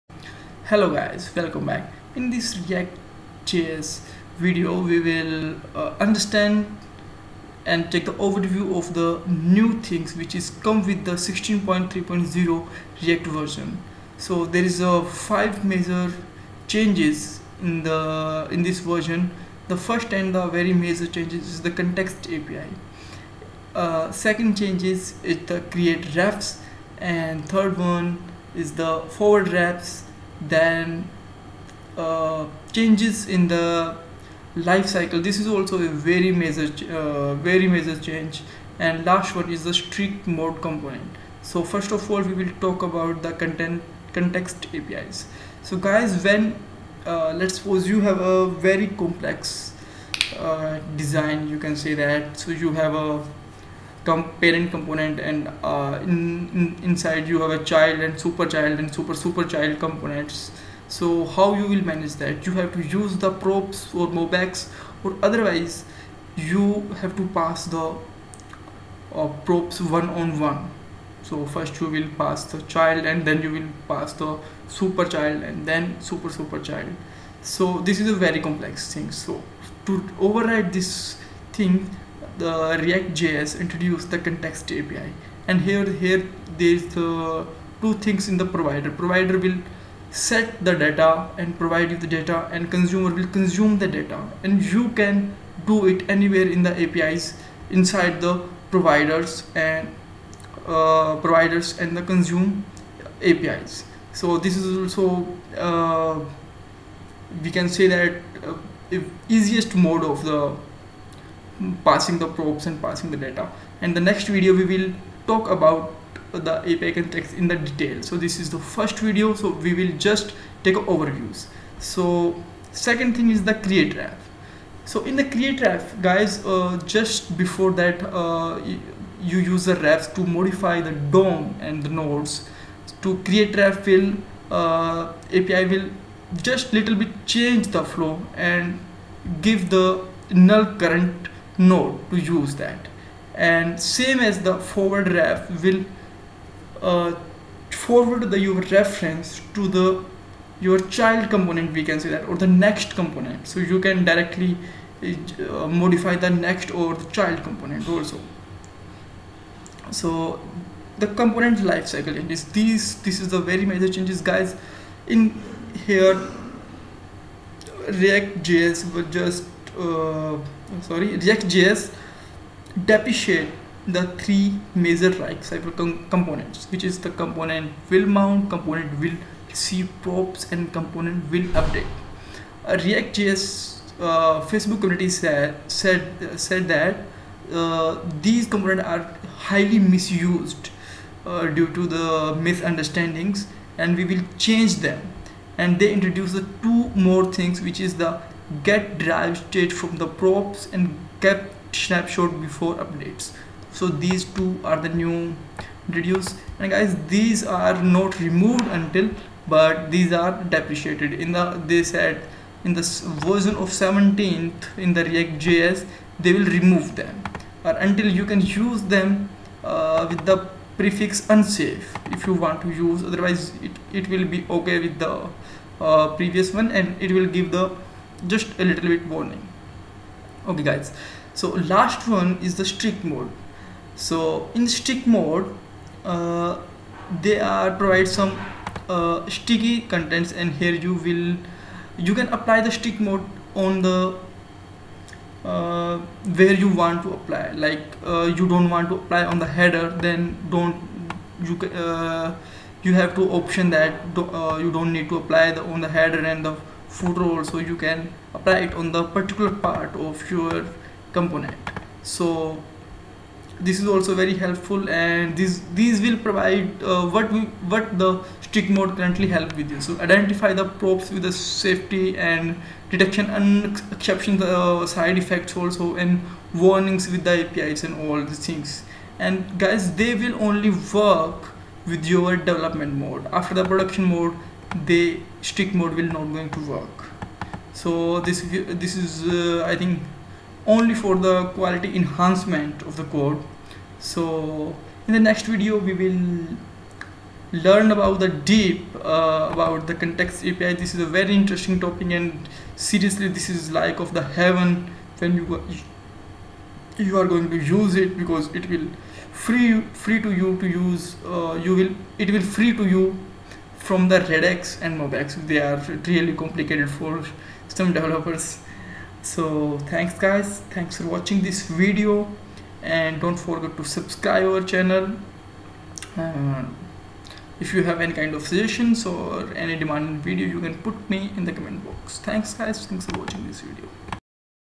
React Js tutorial - overview Of react js 16.3.0
in this video we will learn about react js version 16.3.0 and take a overview of Context api, create refs api, forward refs , change in life cycle and strict mode component etc,
we understand the all new things which comes with this version